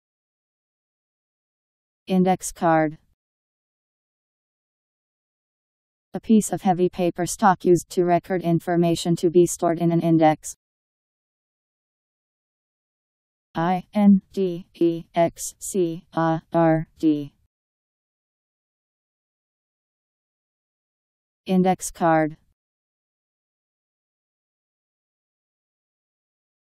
Index card Meaning
Video shows what index card means. A piece of heavy paper stock used to record information to be stored in an index..  Index card Meaning. How to pronounce, definition audio dictionary. How to say index card. Powered by MaryTTS, Wiktionary